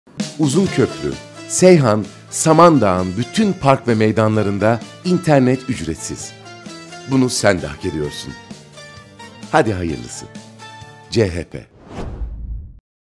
PARK VE MEYDANLARDA ÜCRETSİZ İNTERNET
Uzunköprü, Seyhan, Samandağ’ın bütün park ve meydanlarında internet ücretsiz. Bunu sen de hak ediyorsun.Haydi hayırlısı.